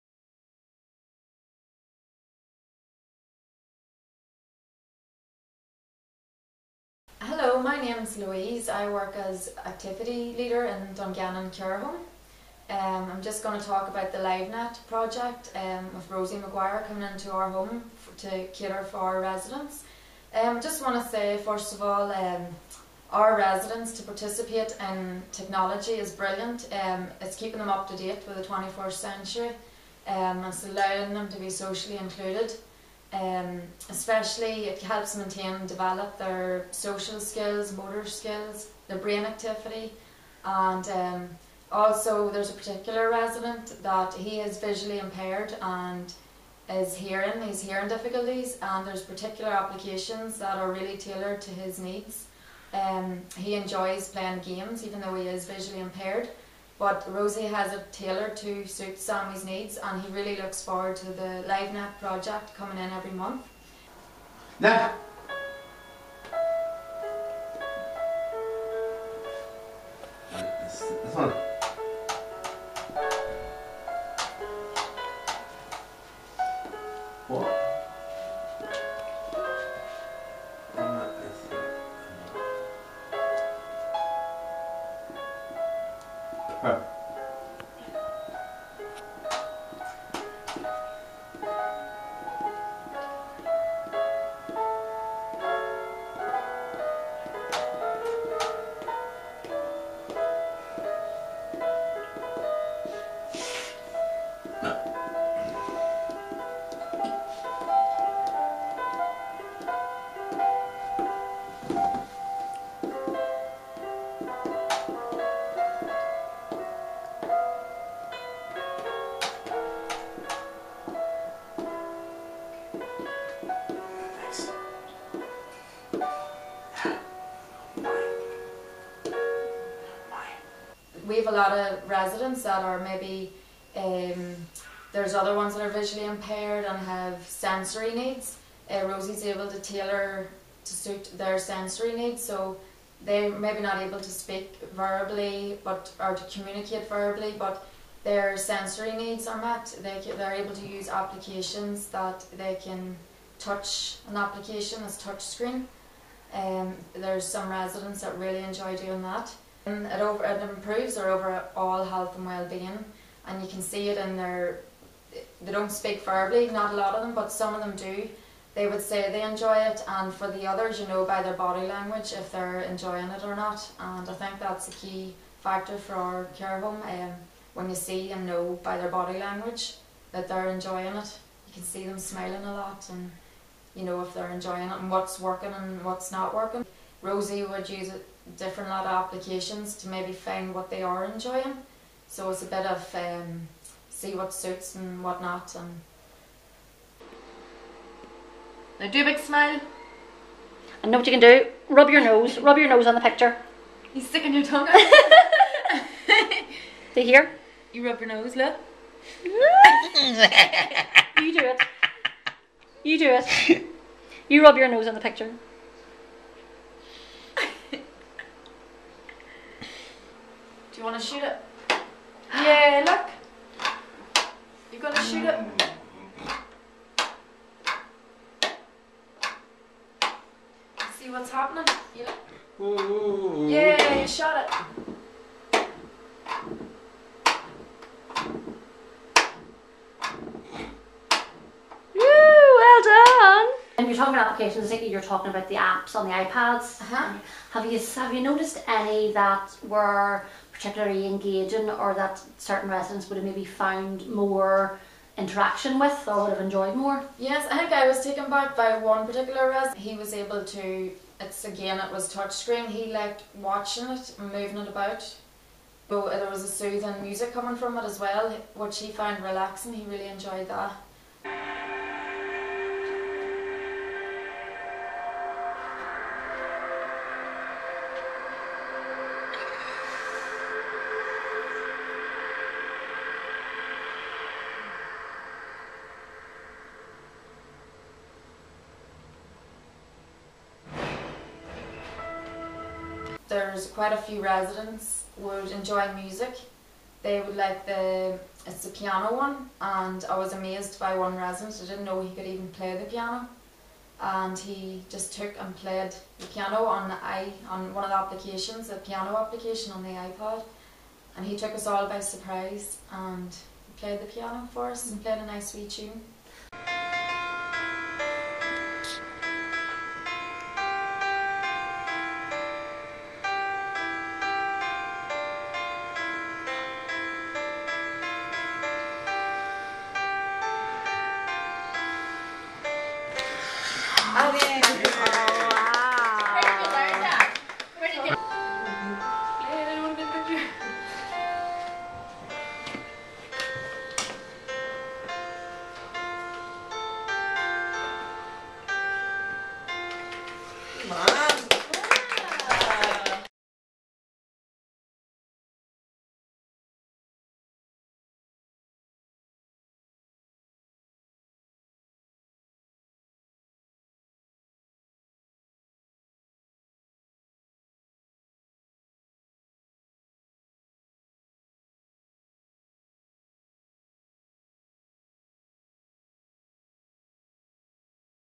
LiveNet Assistive Technology workshops Dungannon Care Home part 1 of 9
Mencap Northern Ireland's LiveNet Project has been delivering ICTworkshops with residents from the Four Seasons Care Home in Dungannon Co. Tyrone.

Have a look at the video's attached to see how i-pads and the Assistive Technology has been used with people with profound and multiple learning disabilities.